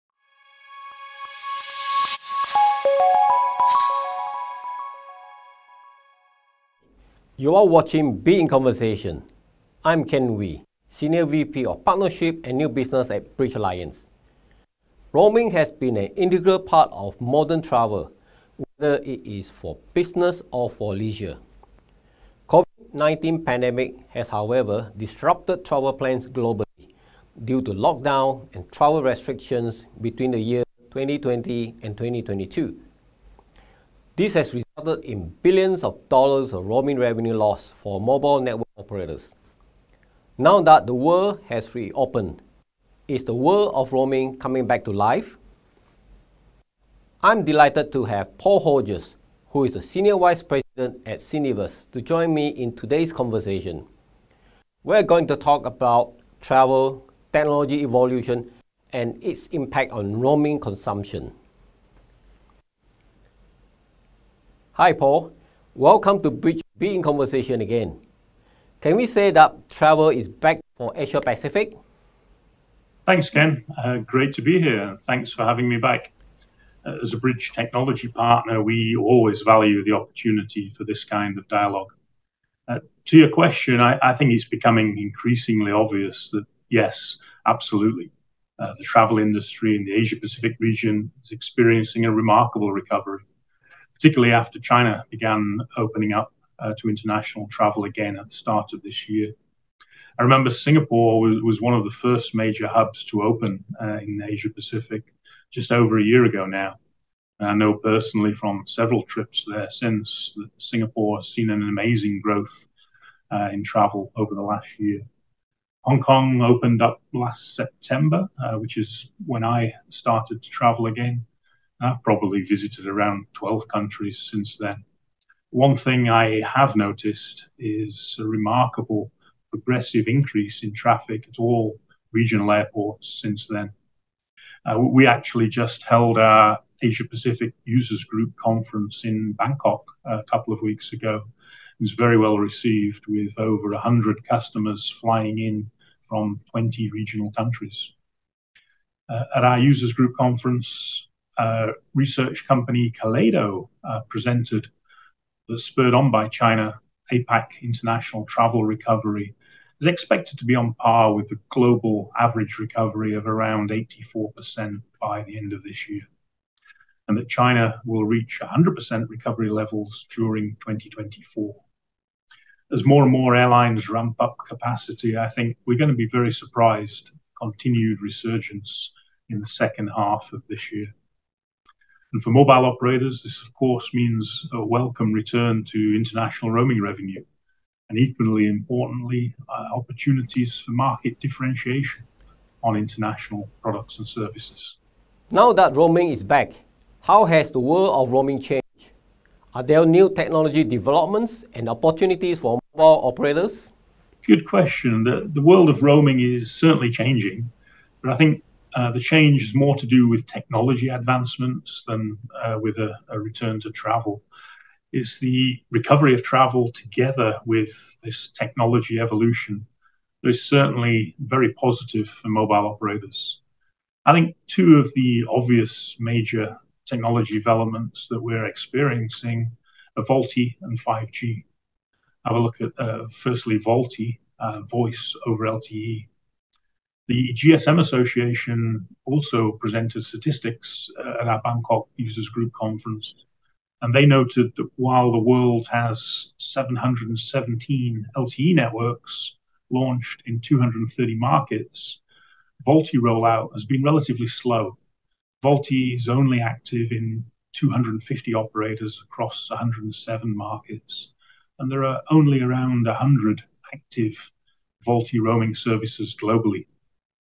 The Revival of Roaming: Bridge Alliance Be in Conversation with Paul Hodges
The pandemic disrupted travel and resulted in significant losses in roaming revenue for mobile operators worldwide. Now changes are happening in the world of roaming at lightning speed.

Check out this insightful interview with our APAC SVP, Paul Hodges.